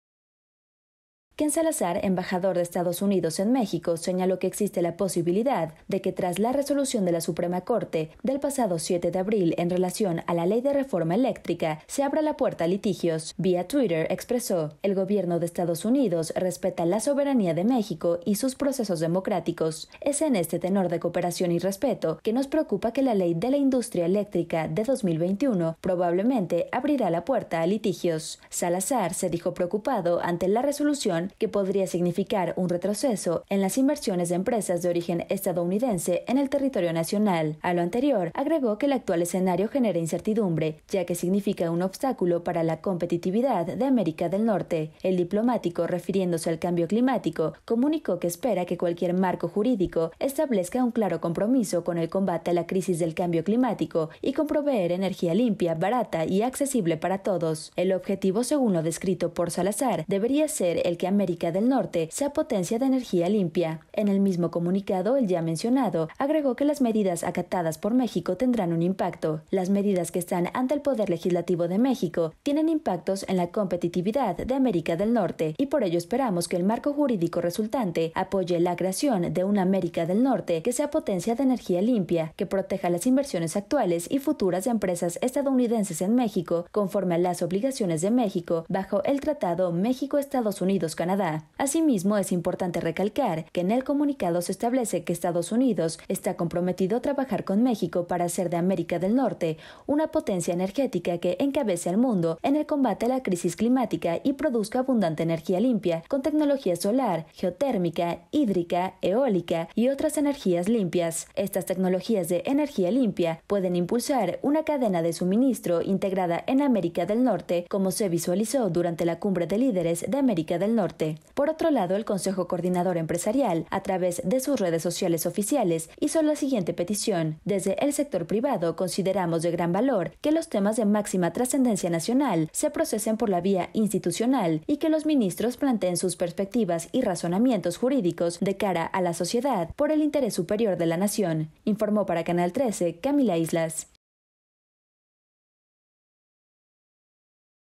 “Preocupa que la ley de la industria eléctrica de 2021 abra puerta a litigios”, declaró ken salazar
Entérate 🔴 “Nos preocupa que la ley de la industria eléctrica de 2021 probablemente abrirá la puerta a litigios”, declaró ken salazar, embajador de EstadosUnidos en México.